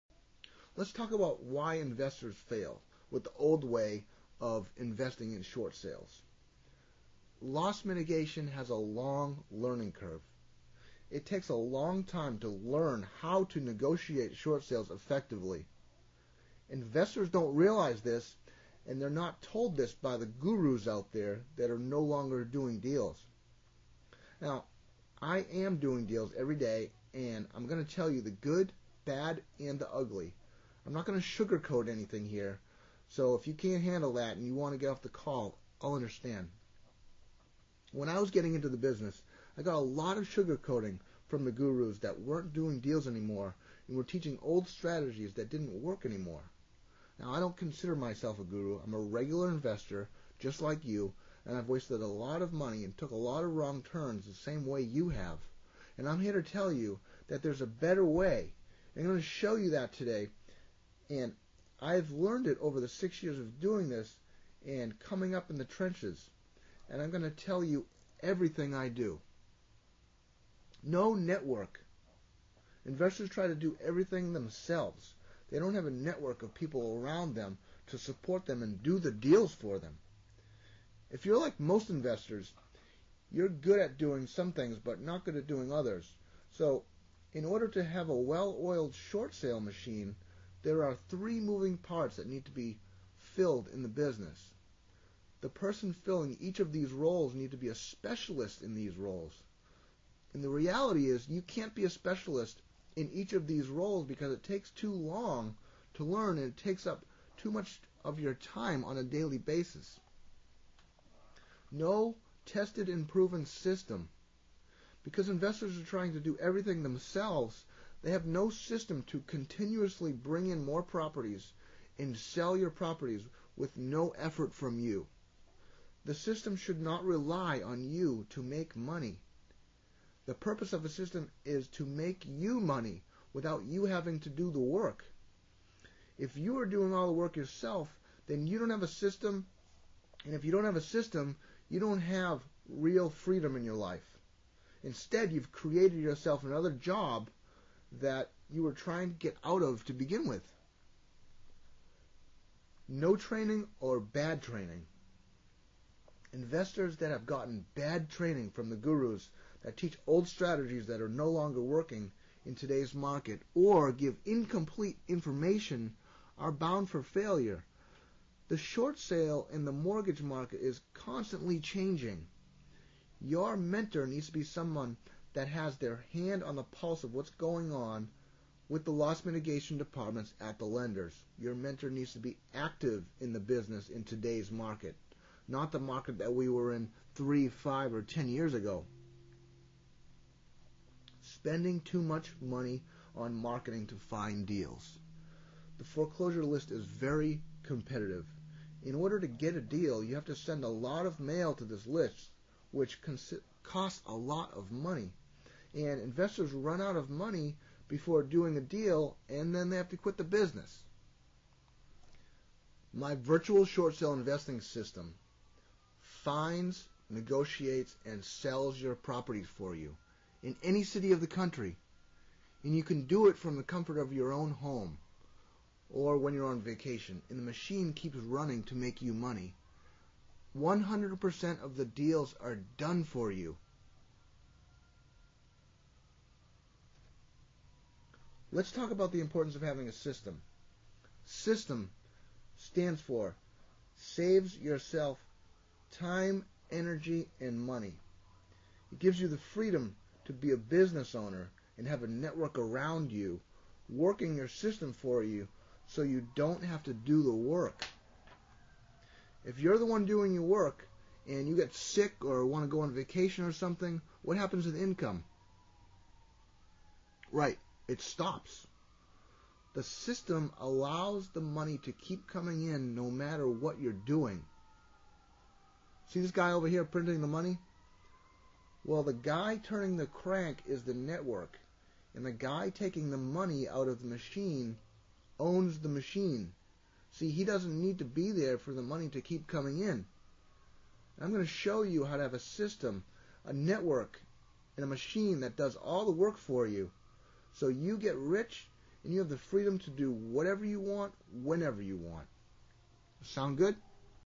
Flipping High-End Short Sales
One of the Hottest Short Sale pros on the planet led this training video.  On it, he shows you exactly how hes making a killing flipping short sales in this market